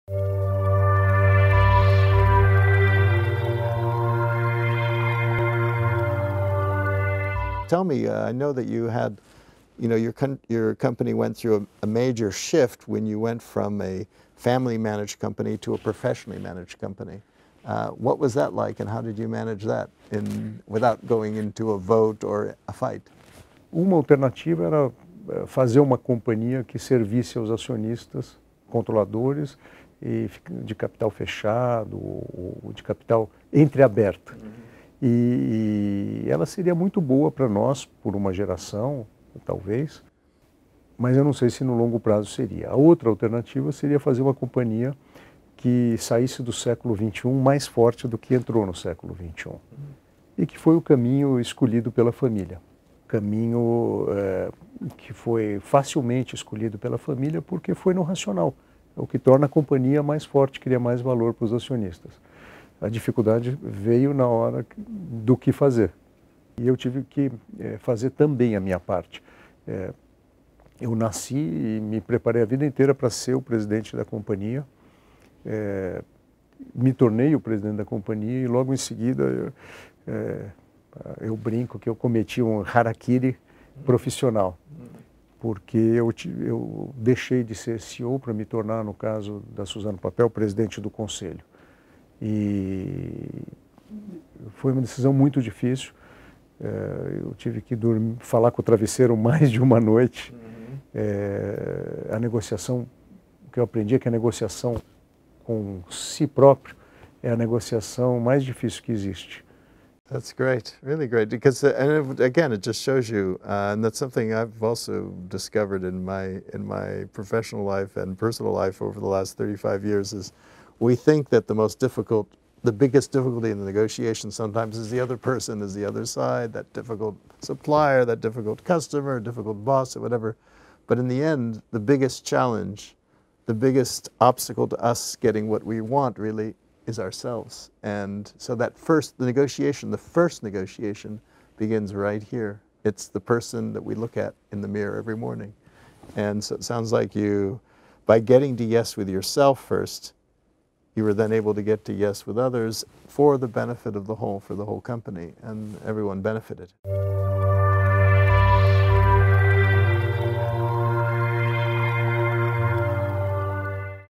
Negotiation in family business | David Feffer and William Ury | short version
Interview by William Ury, Harvard’s professor and expert in mediation, David Feffer talks about negotiation in family business and the challenges faced during Suzano’s professionalization. Check this exchange of experiences.